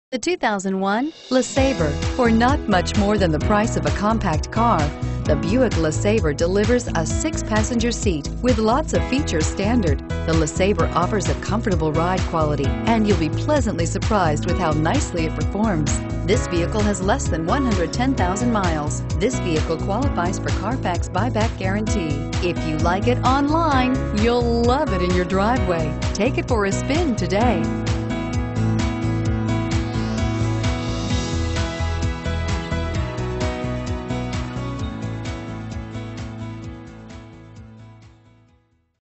2001 Buick LeSabre Duluth GA 307602Y
Acura Carland
3403 Satellite Blvd.

Pick up this WHITE 2001 Buick LeSabre, available today at Acura Carland. Featuring Automatic transmission, it has 109648 miles. This could be the one you've been searching for. Contact the dealer at 1-800-CARLAND and get behind the wheel today!